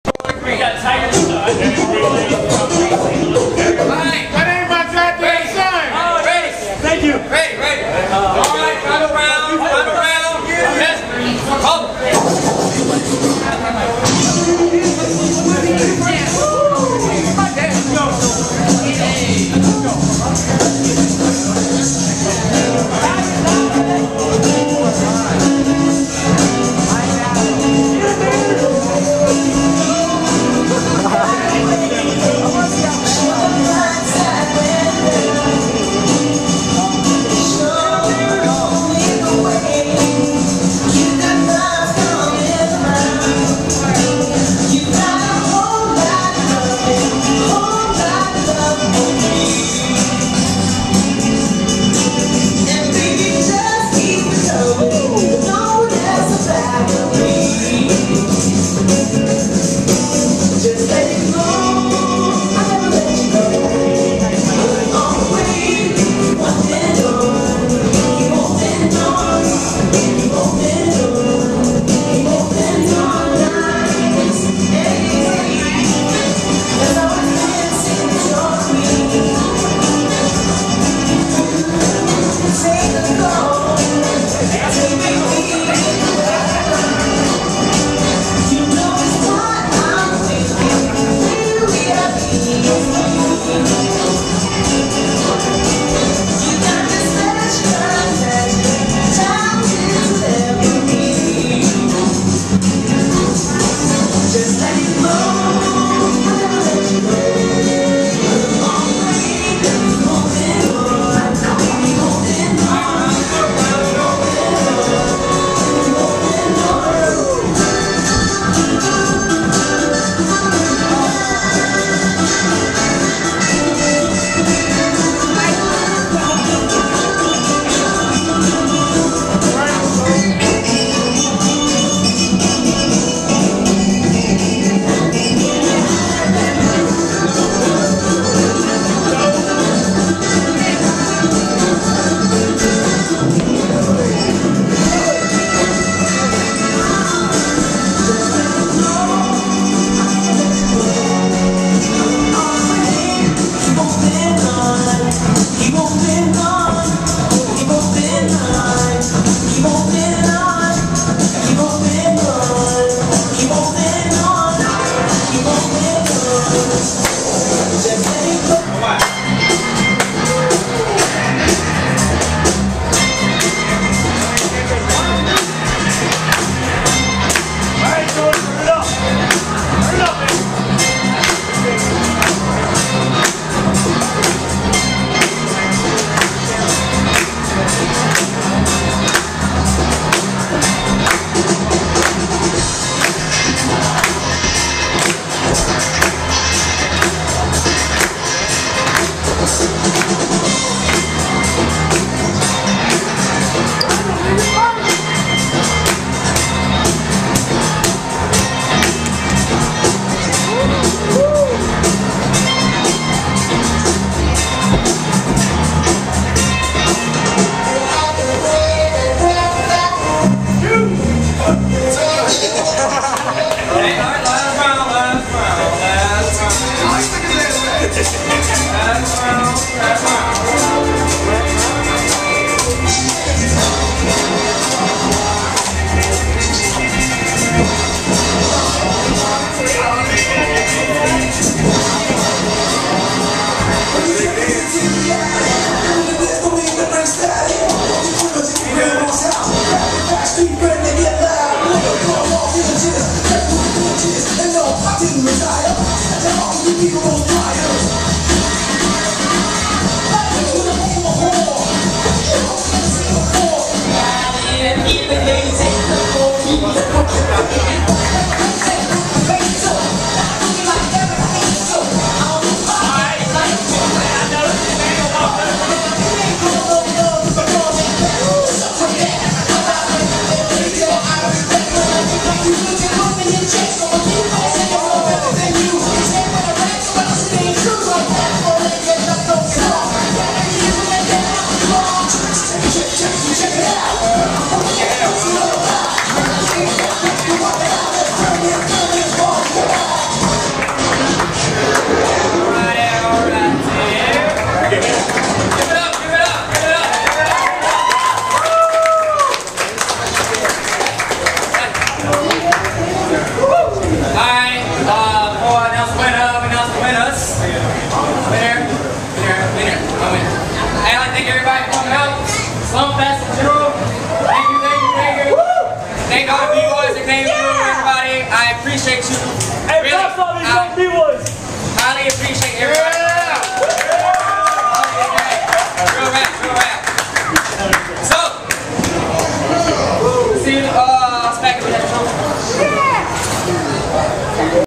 Snapz vs Shoulders @ S.L.U.M. Fest 2012
The final 1 on 1 bboy battle of the day @ S.L.U.M. Fest 2012